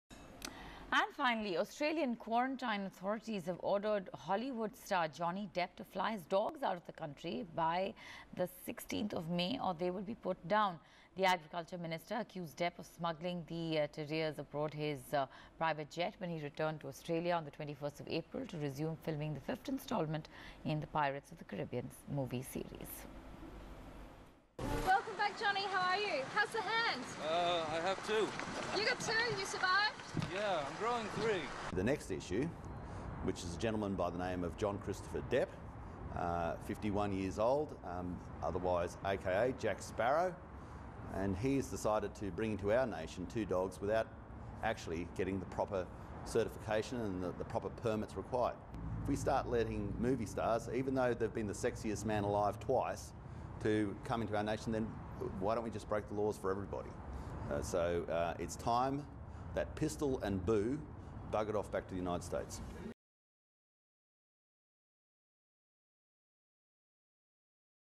Australian Minister ticks off Depp's dogs
Australian Quarantine Authorities have ordered Hollywood star Johnny Depp to fly his dogs out of the country by the 16th of May or they would be put down. The agriculture minister accused Depp of smuggling the dogs to Oz in his private jet when he returned to Australia on 21st April to resume the 5th instalment of the movie Pirates of the Caribbean's 5th series.